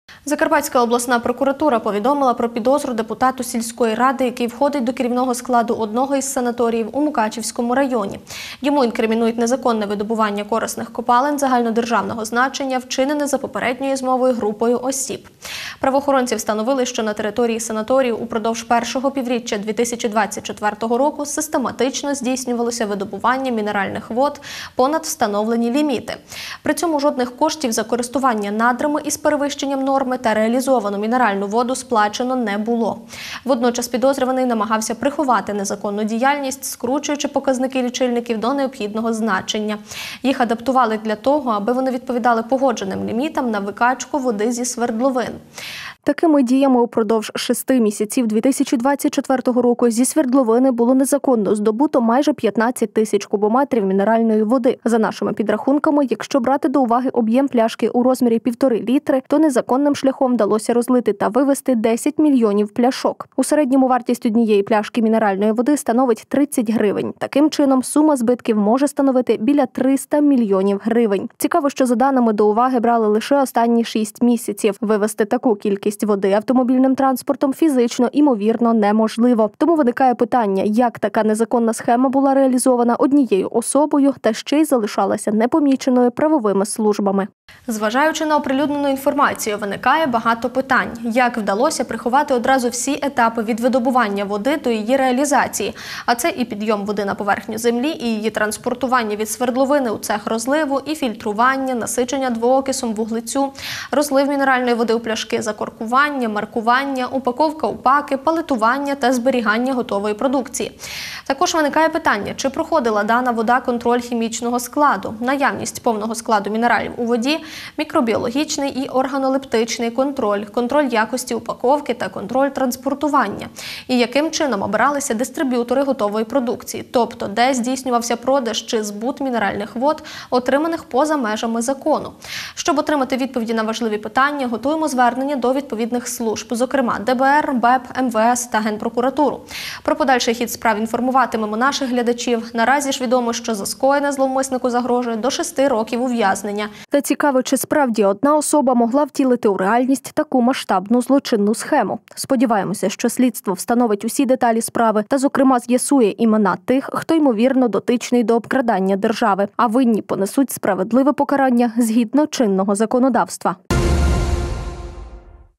Незаконно видобував мінеральні води
Закарпатська обласна прокуратура повідомила про підозру депутату сільської ради, який входить до керівного складу одного із санаторіїв у Мукачівському районі. Йому інкримінують незаконне видобування корисних копалин загальнодержавного значення, вчинене за попередньою змовою групою осіб (ч. 3 ст. 240 КК України). Правоохоронці встановили, що на території санаторію упродовж першого півріччя 2024 року систематично здійснювалося видобування мінеральних вод понад встановлені ліміти. При цьому жодних коштів за користування надрами із перевищенням норми та реалізовану мінеральну воду сплачено не було. Водночас підозрюваний намагався приховати незаконну діяльність, скручуючи показники лічильників до необхідного значення. Їх адаптували для того, аби вони відповідали погодженим лімітам на викачку води зі свердловин. Такими діями упродовж шести місяців 2024 року зі свердловини було незаконно здобуто майже 15 тис. кубометрів мінеральної води. За нашими підрахунками, якщо брати до уваги об’єм пляшки у розмірі 1,5 л, то незаконними шляхом вдалося розлити та вивезти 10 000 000 пляшок. У середньому вартість 1 пляшки мінеральної води становить 30 гривень.  Таким чином, сума збитків може становити біля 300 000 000 гривень. Цікаво, що за даними до уваги брали лише останні 6 місяців. Вивезти таку кількість води автомобільним транспортом фізично, імовірно, неможливо. Тому виникає питання – як така незаконна схема була реалізована однією особою та й ще залишалася непоміченою правовими службами?  Зважаючи на оприлюднену інформацію, виникає багато питань: як вдалося приховати одразу всі етапи від видобування води до її реалізації, а це і підйом води на поверхню землі і її транспортування від свердловини в цех розливу; фільтрування, насичення двоокисом вуглецю (для газованої води); розлив мінеральної води в пляшки; закорковування; маркування; упаковка в паки; палетування (укладання на піддони) та зберігання готової продукції. Також виникає питання чи проходила дана вода контроль хімічного складу (наявність повного складу мінералів у воді), мікробіологічний і органолептичний контроль, контроль якості упаковки та контроль транспортування. І яким чином обиралися дистриб’ютори готової продукції , тобто де здійснювався продаж чи збут мінеральних вод, отриманих поза межами закону. Щоб отримати відповіді на важливі питання готуємо звернення до відповідних служб, зокрема ДБР, БЕБ, МВС та Генпрокуратуру. Про подальший хід справ інформуватимемо наших глядачів. Нараз ж відомо, що за скоєне зловмиснику загрожує до 6 років ув'язнення. Та цікаво, чи справді одна особа могла втілити у реальність таку масштабну злочинну схему? Сподіваємось, що слідство встановить усі деталі справи, та зокрема з’ясує імена тих, хто, імовірно, дотичний до обкрадання держави. А винні понесуть справедливе покарання згідно чинного законодавства.